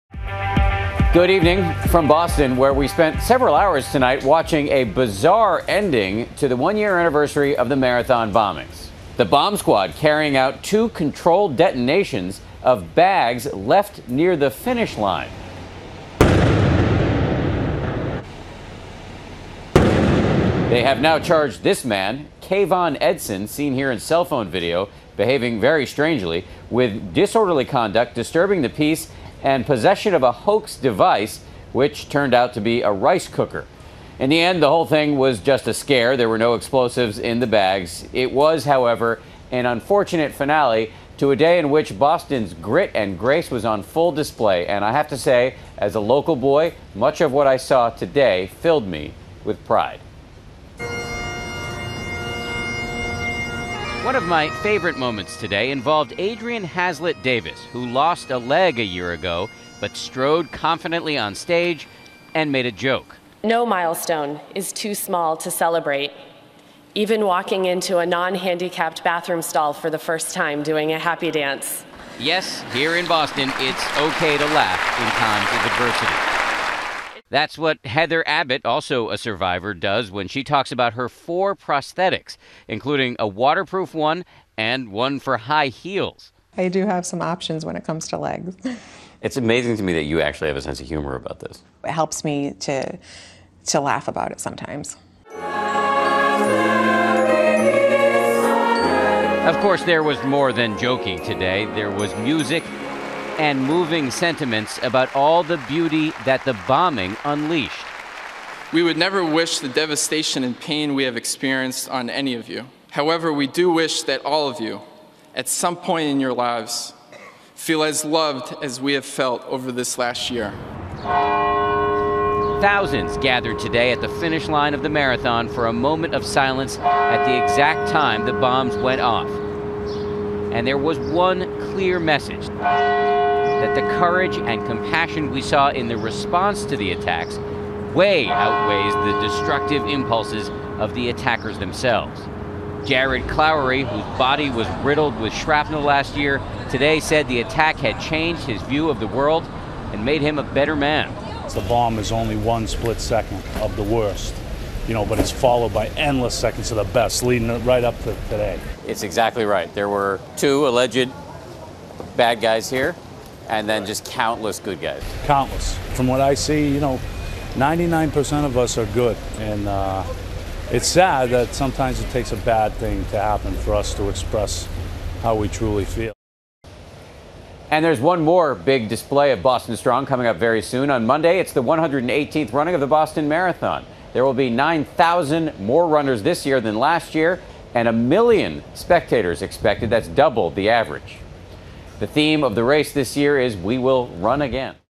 Boston, Stronger Than Ever, One Year After Marathon Bombing
ABC's Dan Harris talks to survivors about how the attack impacted them and their road to recovery.